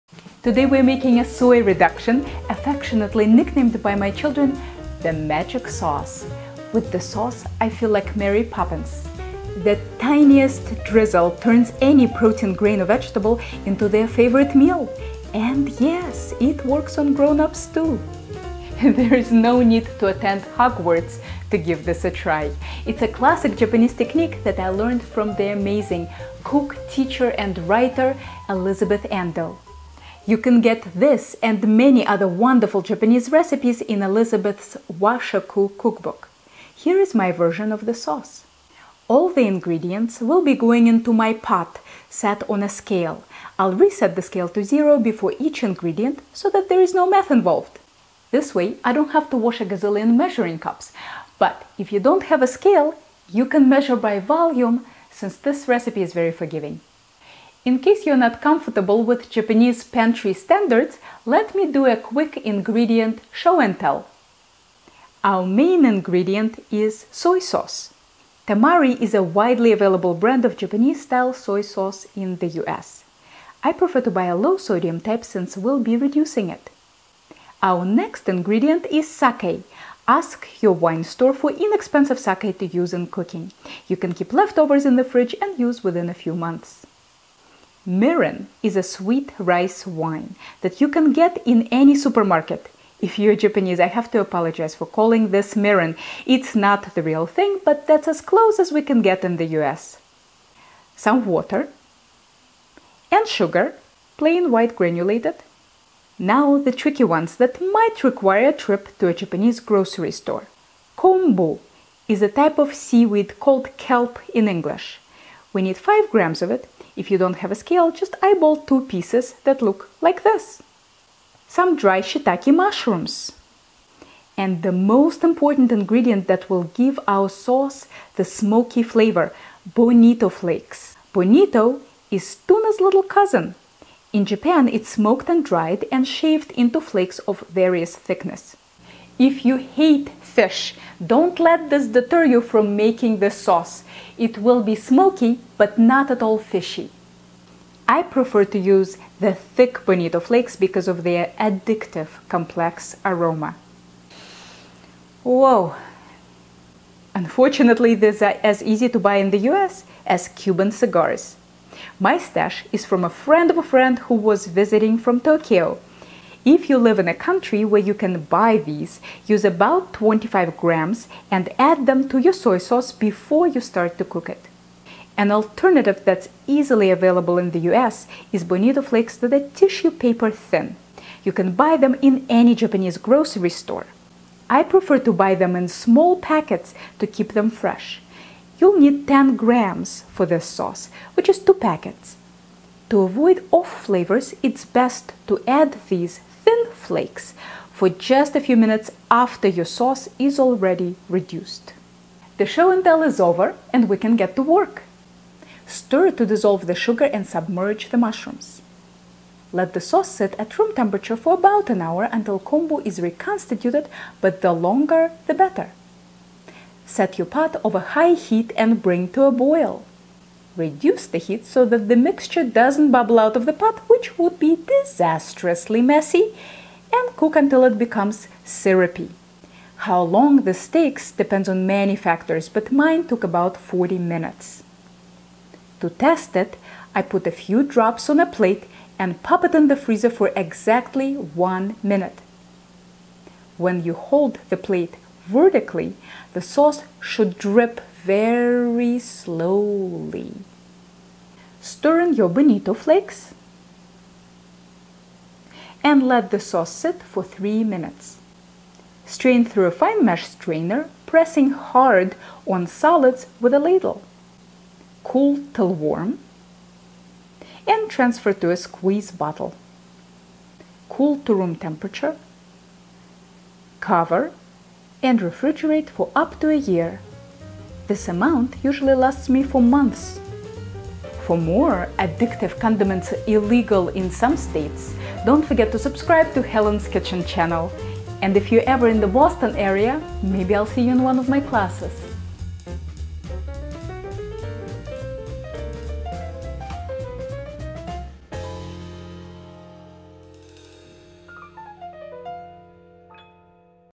Japanese Soy Reduction
Soy Concentrate
(adopted from Washoku Cookbook by Elizabeth Andoh)

5 g kombu (20 square inches)
8 g dry shiitake mushrooms (1/2 cup)
340 g soy sauce (1 and 1/3 cup - that's one 10 oz bottle)
157 g sake (2/3 cup -- buy the real stuff, don’t use “sake for cooking”)
65 g mirin (1/4 cup)
80 g water (1/3 cup)
75 g granulated sugar (1/3 cup, plus 2 tsp)
10 g thin bonito flakes (1 cup lightly packed) (or 25g thick bonito flakes, atsu kezuri)

In a small saucepan, combine everything together except for katsuo-bushi (unless you are using atsu kezuri, in which case, add it right in). Let sit for 1 to 12 hours at room temperature.
Bring to a boil on the stove top and regulate heat so that the mixture bubbles, but doesn’t bubble out of the pot (watch out, this sauce gets foamy, so don’t leave it unattended). Simmer until syrupy. Take off heat.
If using thin bonito flakes, stir them into the sauce and let sit for 3 minutes.
Strain the sauce through a fine mesh strainer forcing it through with the back of a ladle.
Cool until barely warm and move to a jar or squeeze bottle. When the sauce cools completely, cover tightly and store in the fridge. This sauce doesn’t spoil, but is best if used within 3 months.